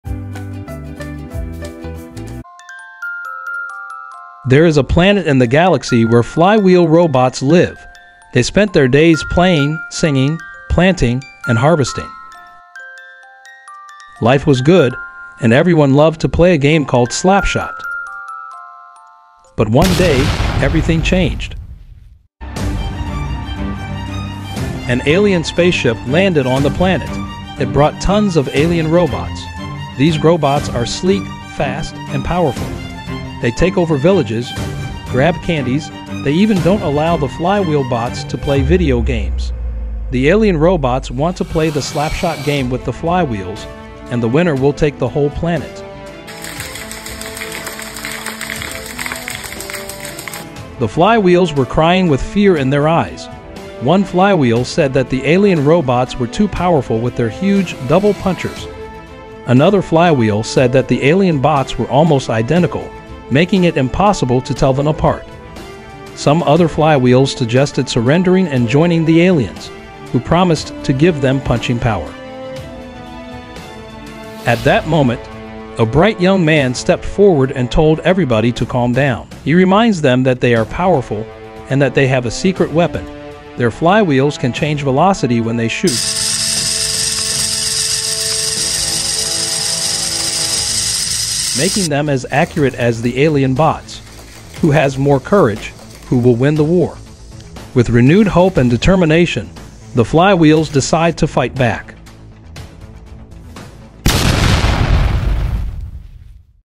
Flywheel vs Alien Bot. Midjourney, AI, VEX IQ Slapshot by Flying Cheese.
Yes, we are trying Midjouney, lol.